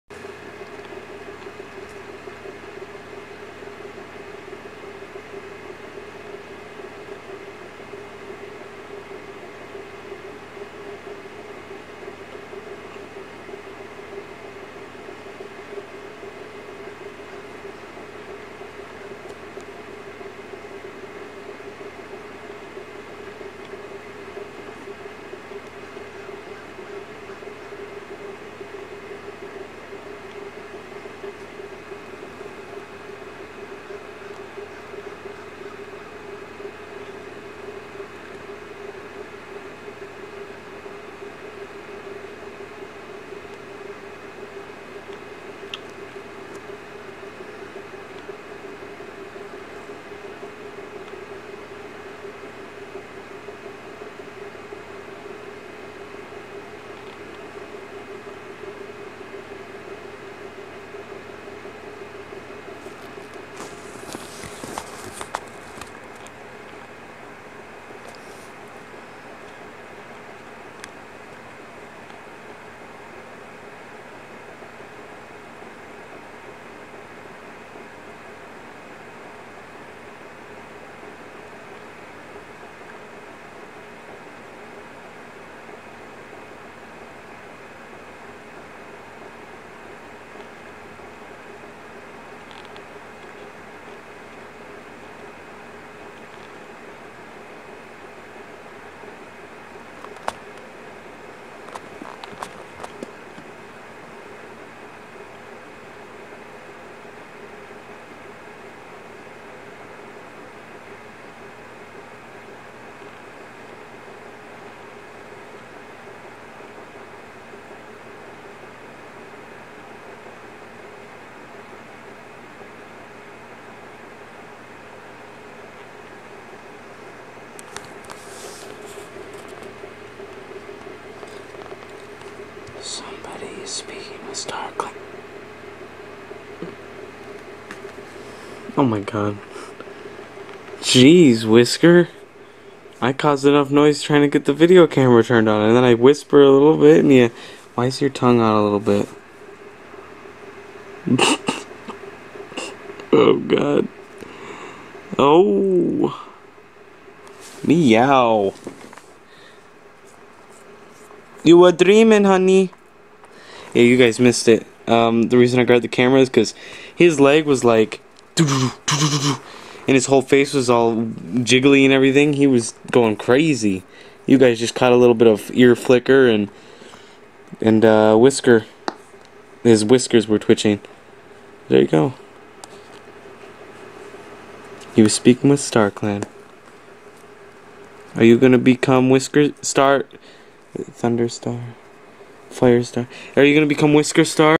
My Cat Whisker Is Dreaming
Hello! Welcome to my first video camera video. This video is actually kind of old. However, I finally got my video camera to work with uploading videos to the computer, so I can post them to YouTube. Planning on uploading a lot of videos now, things like box openings, collections, cat videos, and more.

You can tell this video is old because I refer to Whisker dreaming as "Speaking to StarClan", from the Warriors book series, I haven't touched that series in over a year.

In this video we see my cat Whisker (Short for "RainWhisker") dreaming. I tired to capture the full dream, but sadly you guys missed the best parts. Either way, here he is.